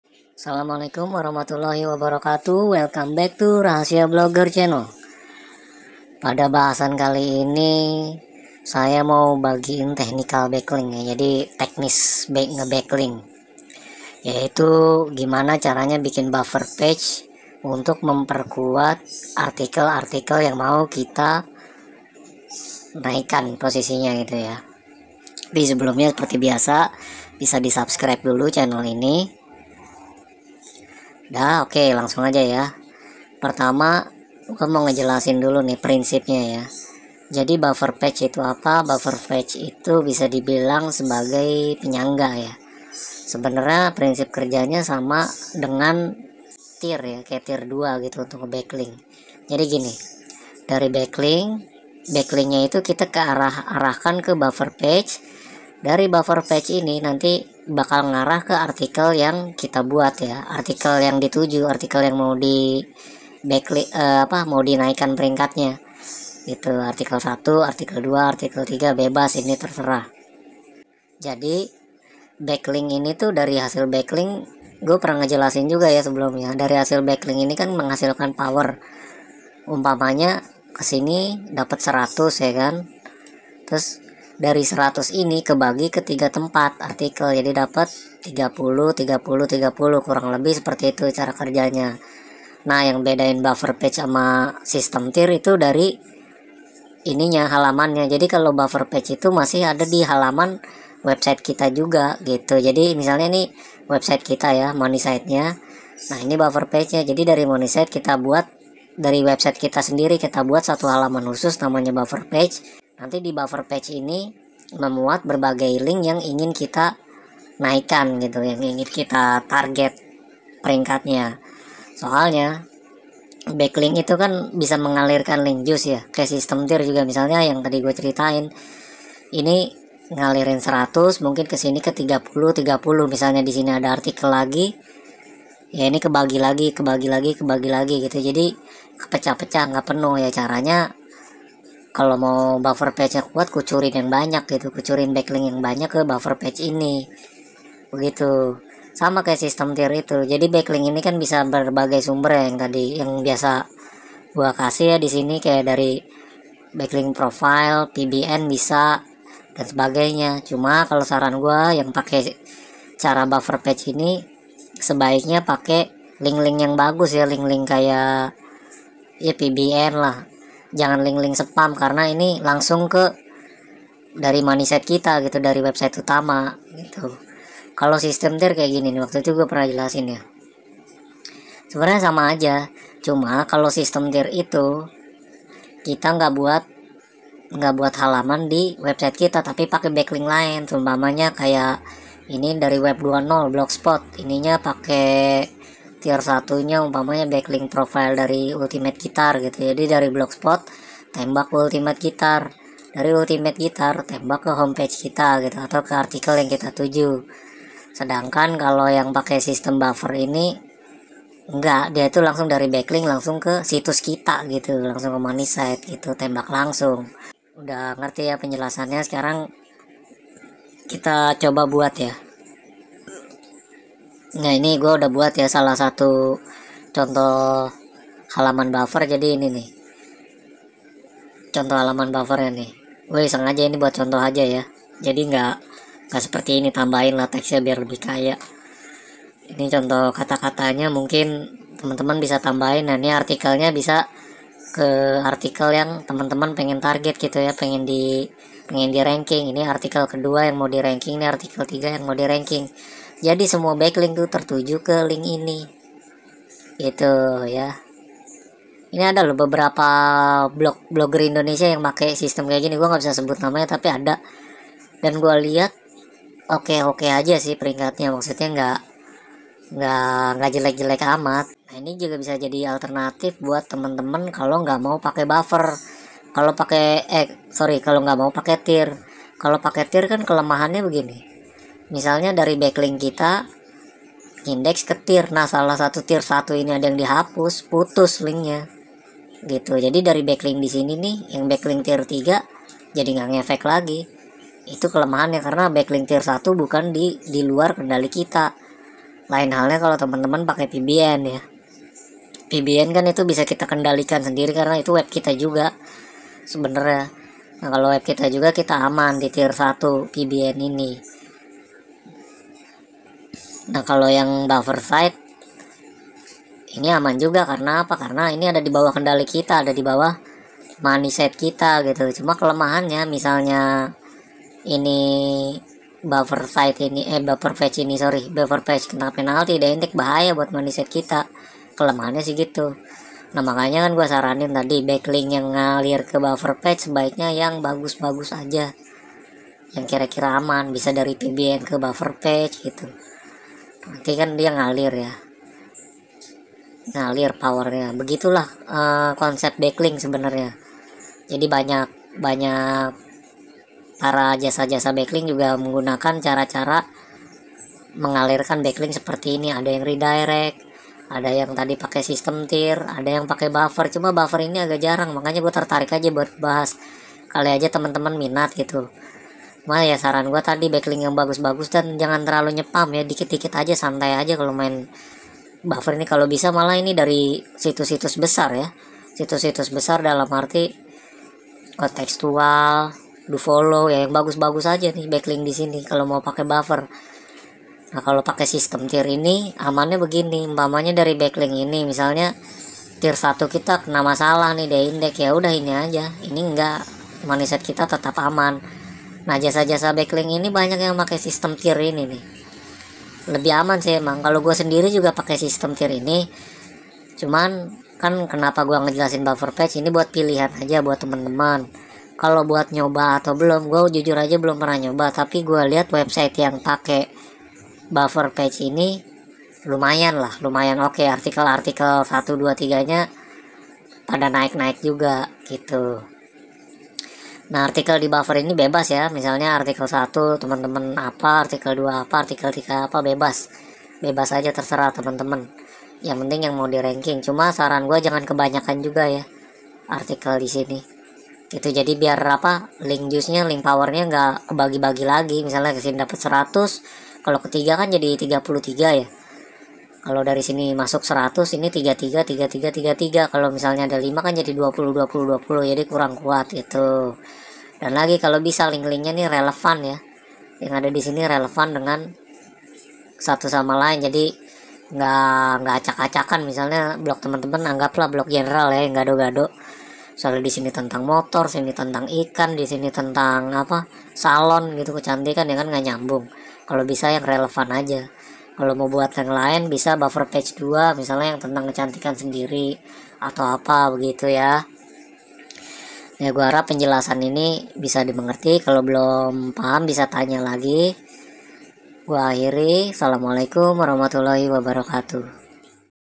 CARA KERJA BACKLINK - BUFFER PAGE
Video ini akan menjelaskan bagaimana cara kerja backlink, terfokus pada buffer page.

Apa saja manfaatnya dan bagaimana penggunaan buffer page? Silahkan simak video sampai habis.

Voice by Pedagang Bakso Borax

Sstttt!!! Diem-diem aja yes!